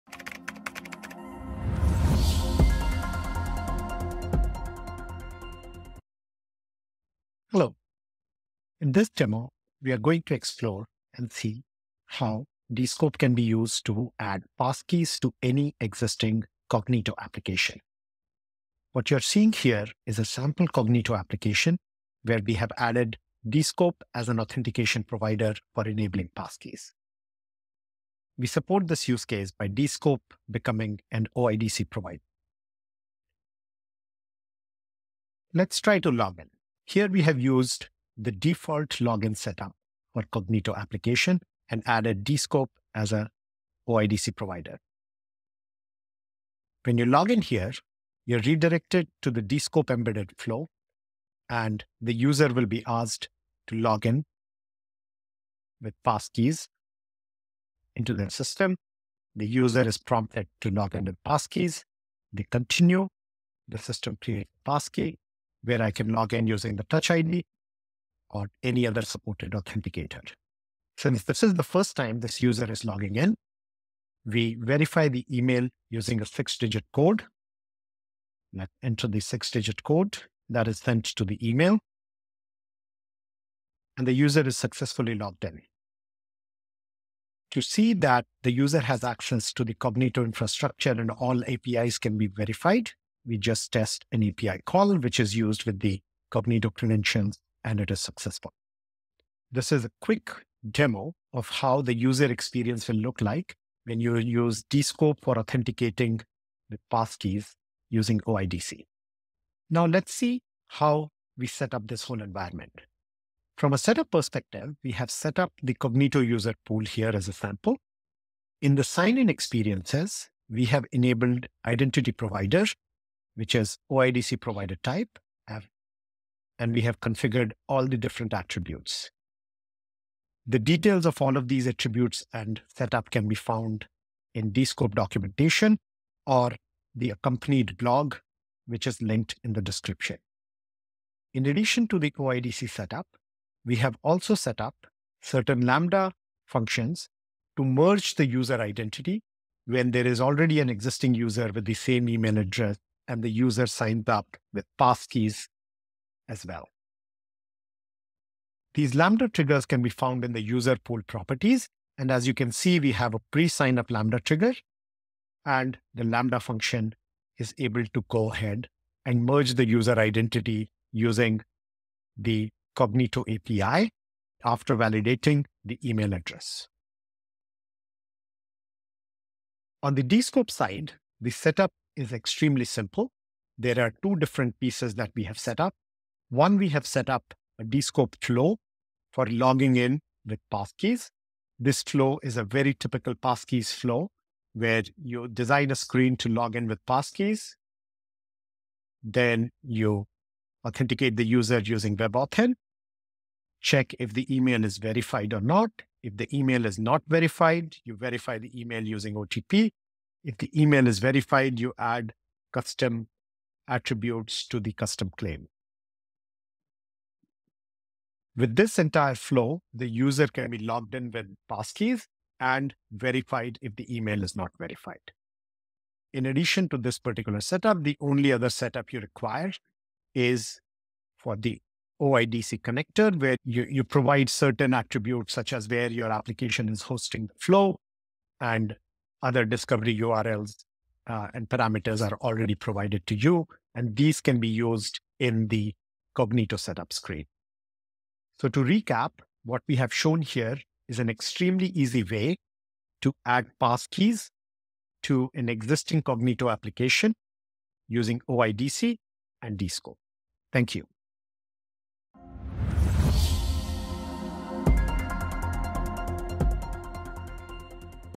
Add Passkeys To Your Amazon Cognito Login With Descope
Learn how you can augment your Amazon Cognito login with passkey authentication by using Descope as a federated identity provider.